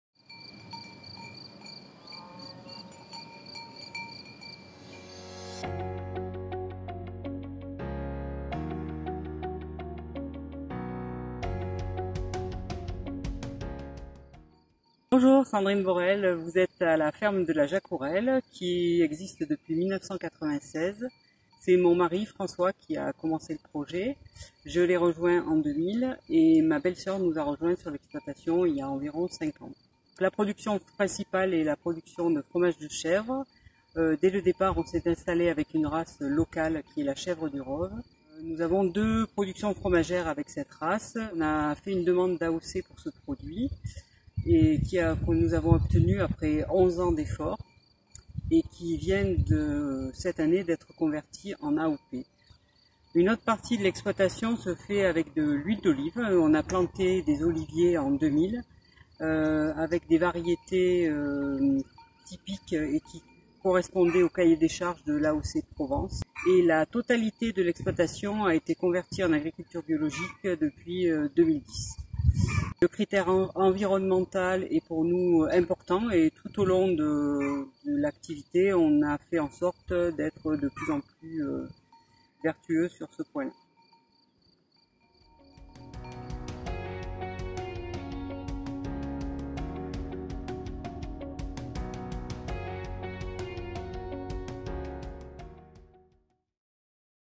Episode 1 - La ferme La Jacourelle
"Je suis producteur à La Roque d'Anthéron", est une série proposée par la Ville et l'Office de tourisme de La Roque d'Anthéron en 2020, suite à la crise COVID-19.

Au fil des épisodes, découvrez des portraits de producteurs locaux, qui vous proposent en vente directe leurs produits issus de notre terroir.
Episode 1 : Sandrine et François Borel font vivre la ferme La Jacourelle. Ils élèvent des chèvres bien particulières et cultivent des oliviers...pour le plus grand plaisir des gourmets.

Regardez la vidéo pour en savoir plus... et partagez !